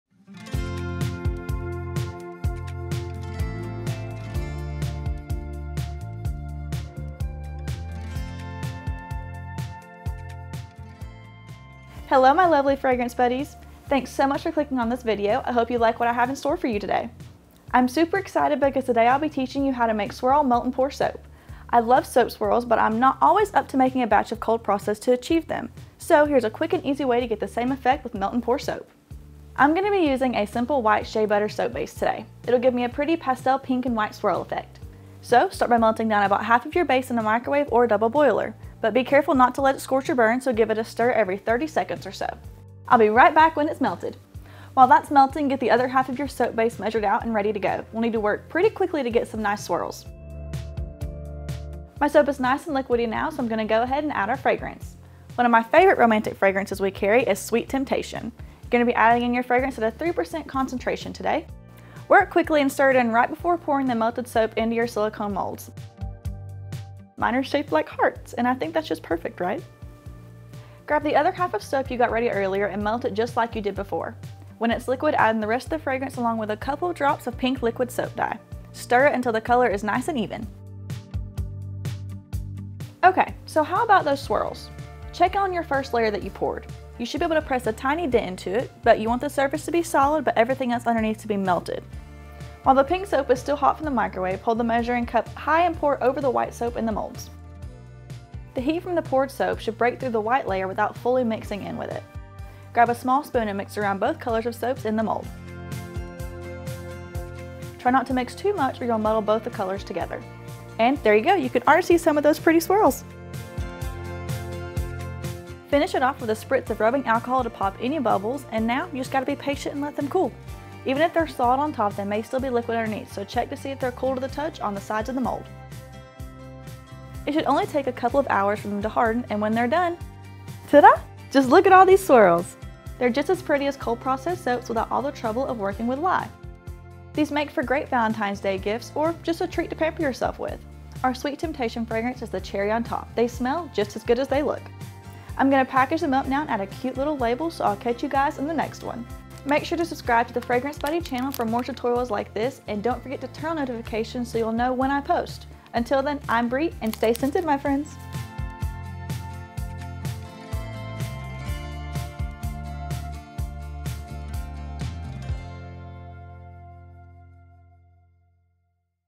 How to Make Swirled Heart Soaps (Melt and Pour)
Hey fragrance buddies! Want to Learn how to make beautiful swirled heart soaps? Watch this simple tutorial and learn how! Perfect for Valentine's Day or any special occasion, these homemade soaps are easy to make and make great gifts for friends and family. Watch and learn how to create your own unique and colorful swirled heart soaps today!

Reed Diffuser Intro: (00:00)
Reed Diffuser Ingredients: (00:05)
Adding Sage: (00:18)
Fragrance Mixture: (00:36)
Combining Mixtures: (01:19)
Adding Reeds: (01:54)
Reed Diffuser Final Product (02:01)